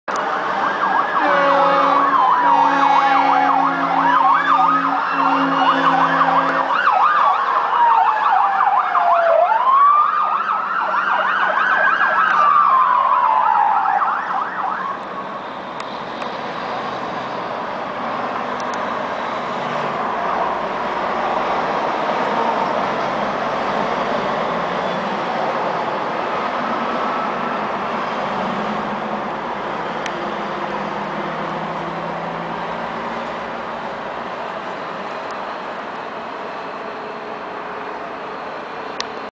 Coralville, IA Police and Johnson Co. Ambulance
Coralville, IA Police and Johnson County Ambulance arrive on the scene of an overturned Roadway semi truck on I-80.